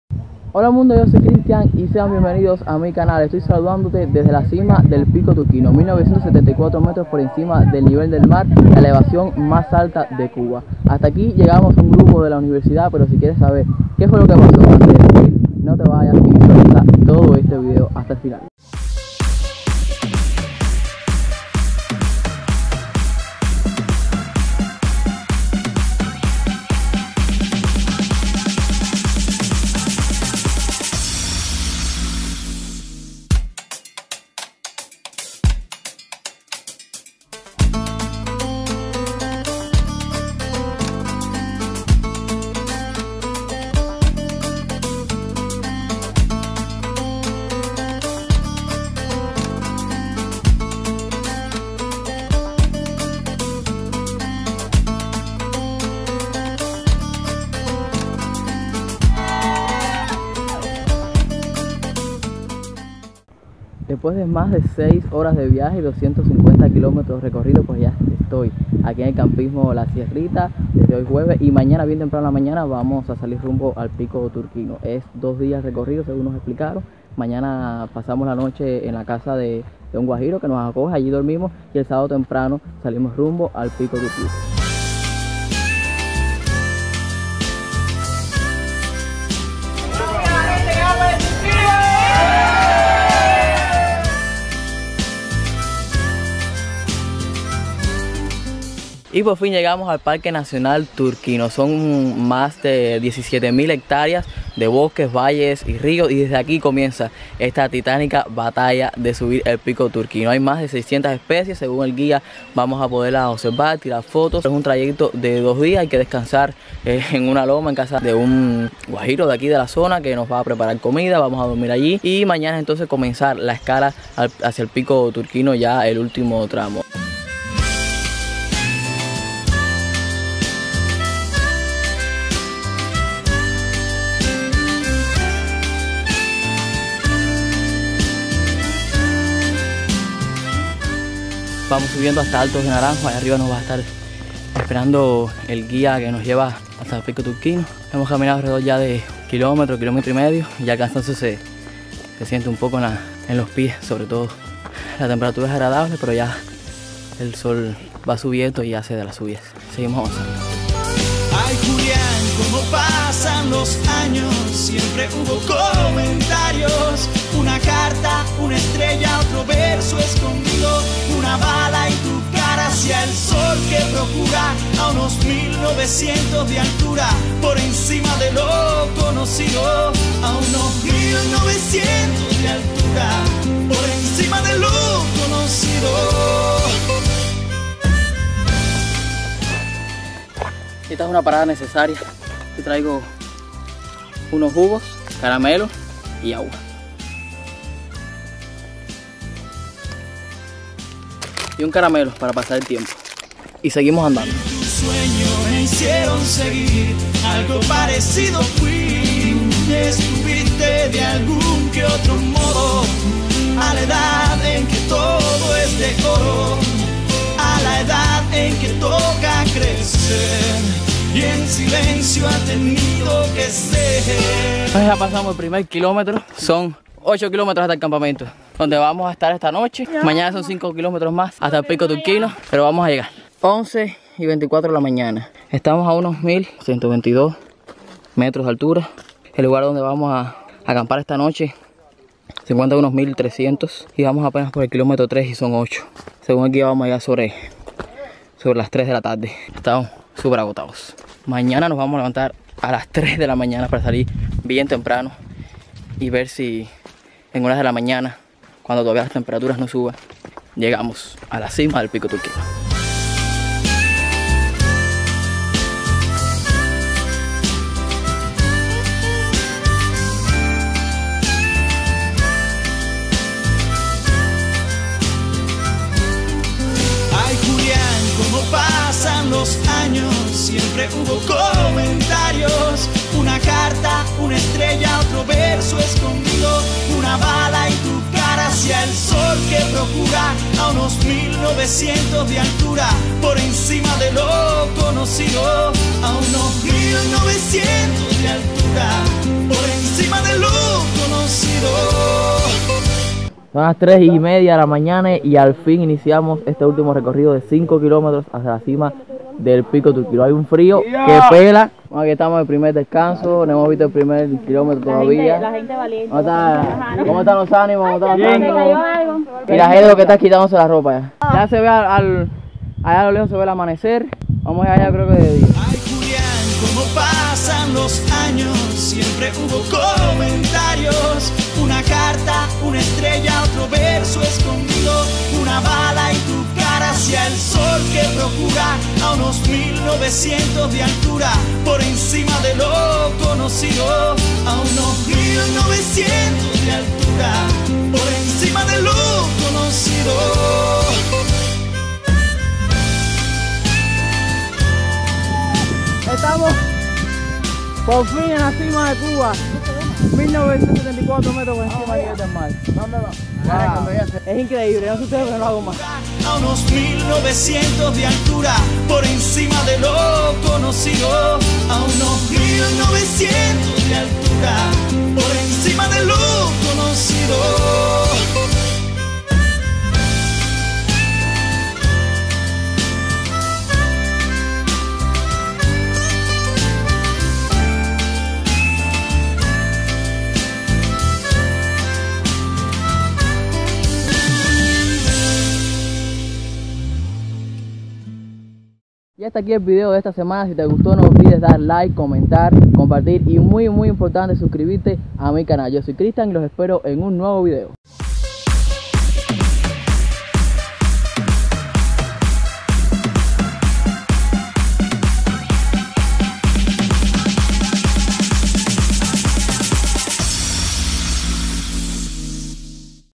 Subí el Pico Turquino!!! ►► Kristian Vlog
Una aventura extraordinaria subir hasta la cima del Pico Turquino, la elevación más alta de Cuba, a 1974 metros sobre el nivel del mar.